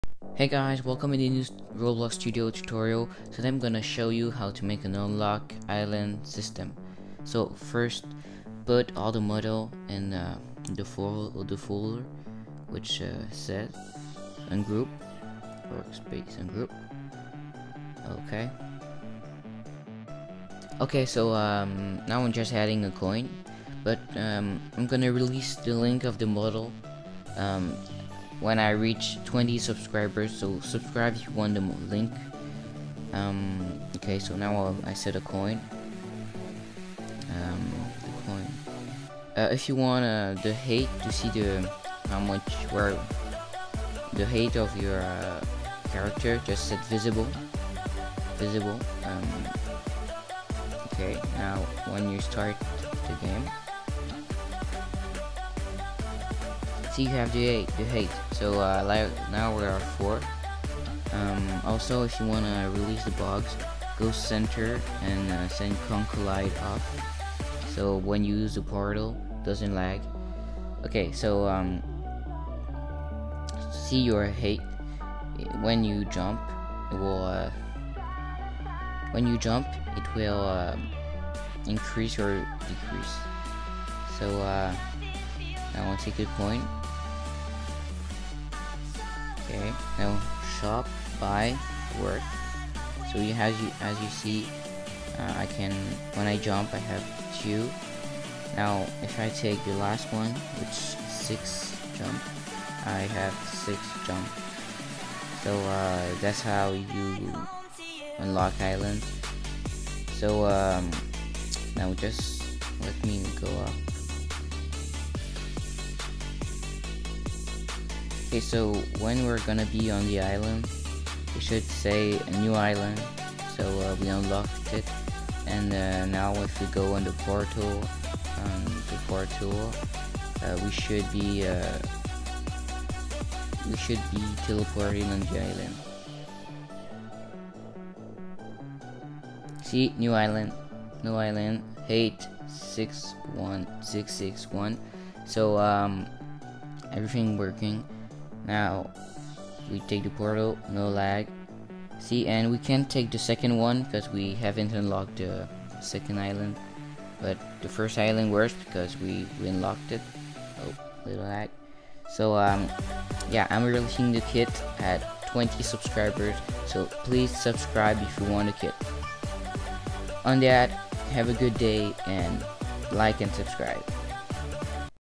how to make Island Unlock System roblox studio!!
Hey guys today i will show you how to make an island unlock system (I made a free model) !! On that have a great day 👍 hope it will be helpful :3
Free model: